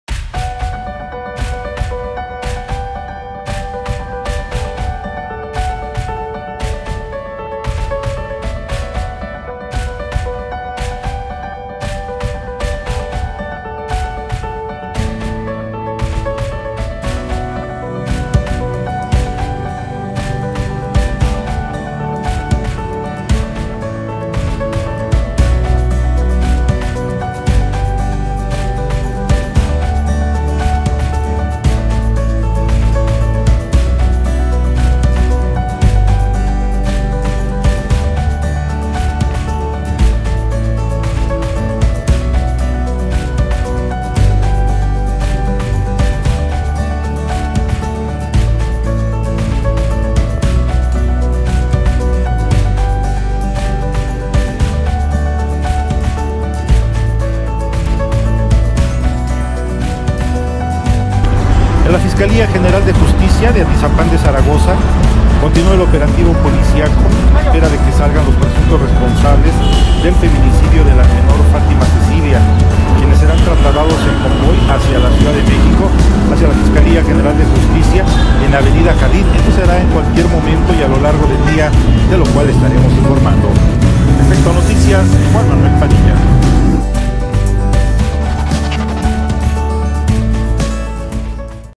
Feminicidio de Fátima: Así fue detenido el asesino y era vigilado por drone
Las autoridades policiales de México difundieron un video que muestra el momento de la detención de Mario Alberto Reyes Nájera, feminicida y asesino de la niña Fátima de 7 años en México.